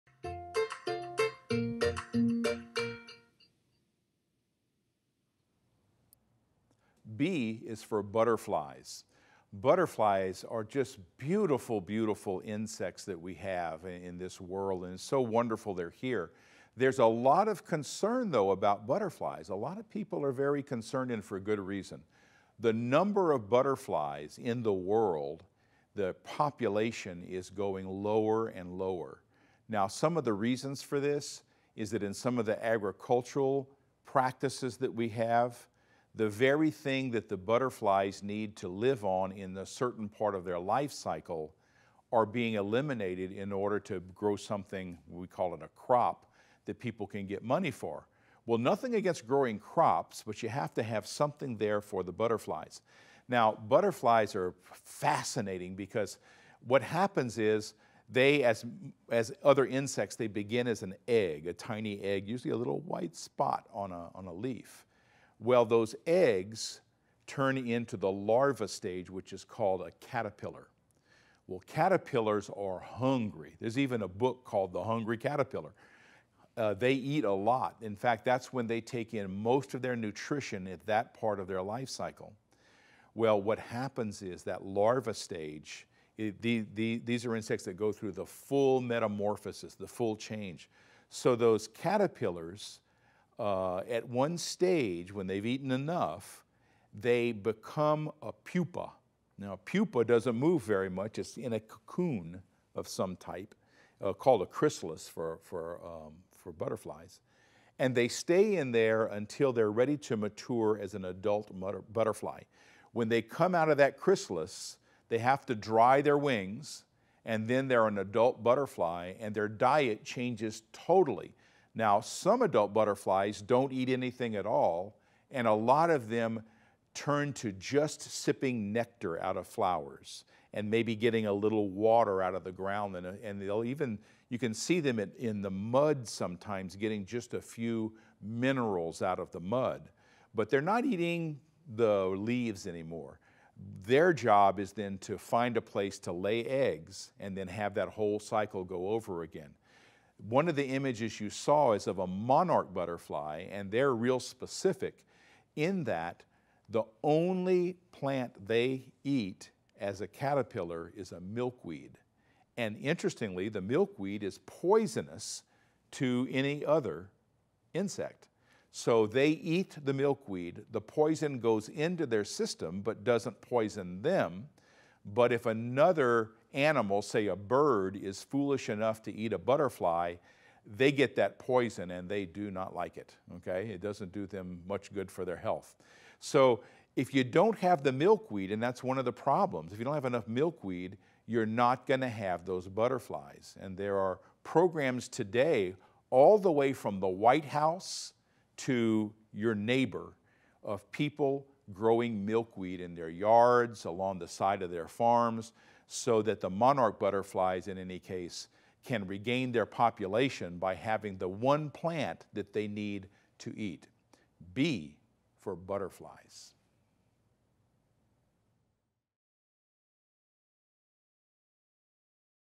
SOCKS SHORT: B is for Butterflies
Butterflies are beautiful and delicate creatures. Learn a little more about them and specially monarchs in this episode of our series of shorts about animals.